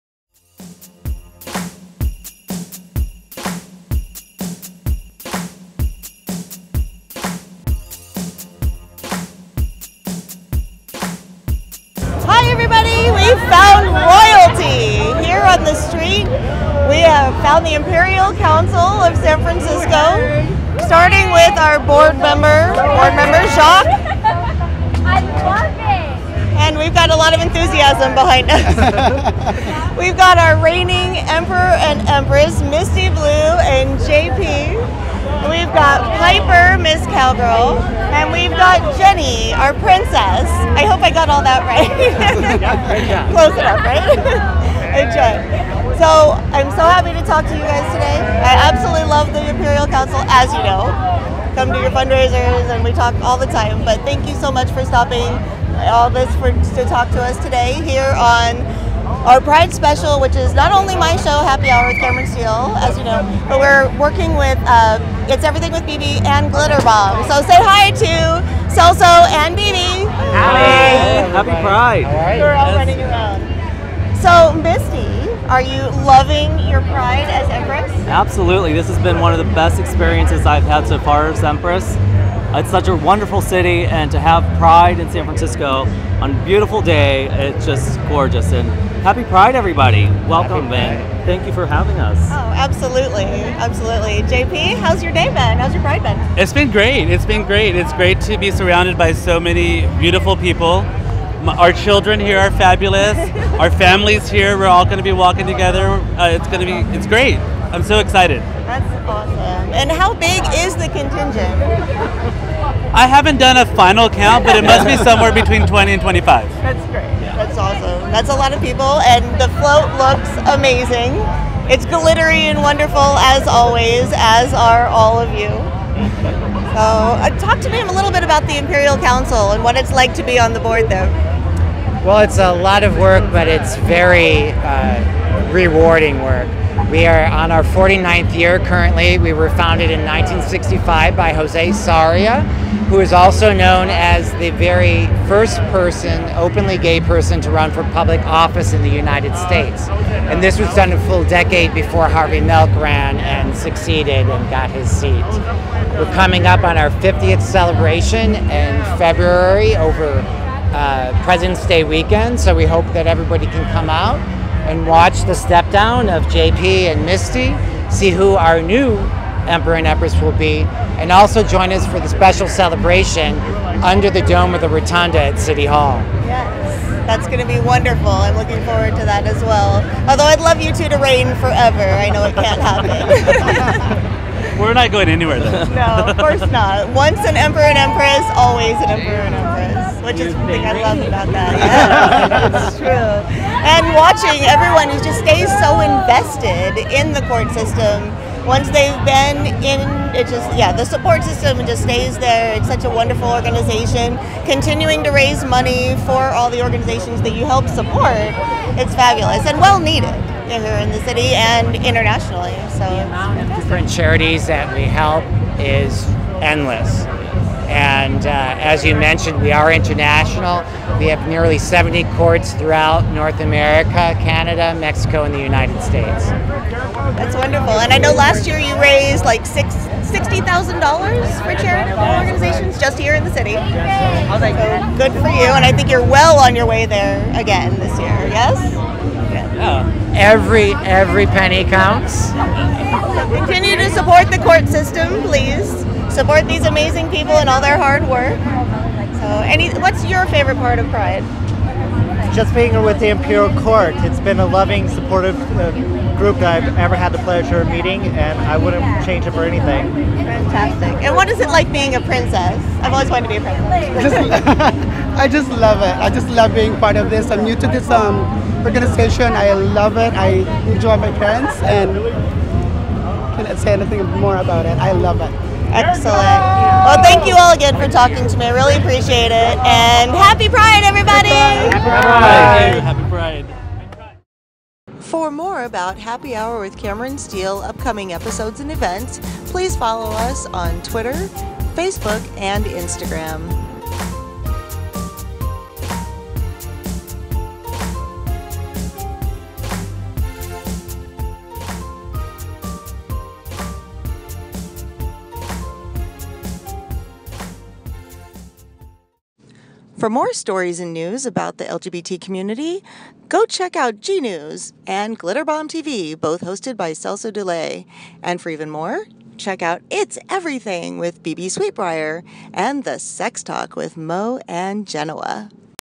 Happy Hour Finds Royalty at Pride 2014!
The second in a series of "Pride Shots" taken behind-the-scenes at San Francisco Pride 2014, Happy Hour with Cameron Stiehl catches up with friends from the Imperial Council of San Francisco as they await the start of the parade! Meet board member and Emperor XXV, Jacque Michaels, reigning
Empress & Emperor Misty Blue and John Paul Soto, Miss Cowgirl Piper Angelique, and Princess Jenny Twoblocksaway and learn a bit of fun San Francisco history!